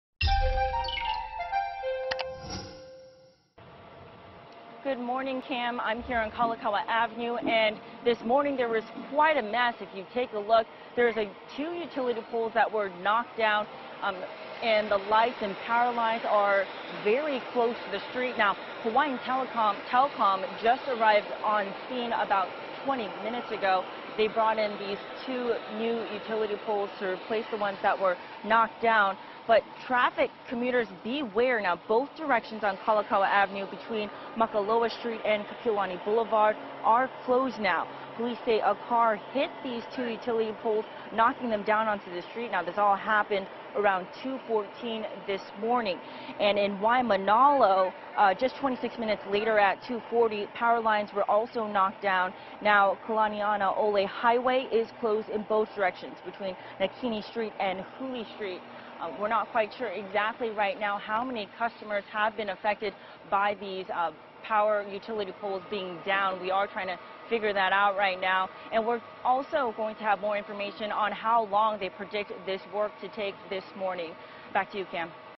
Utility poles knocked down across Oahu
There were two closures on Oahu Thursday morning due to utility poles knocked down. For news you need to know,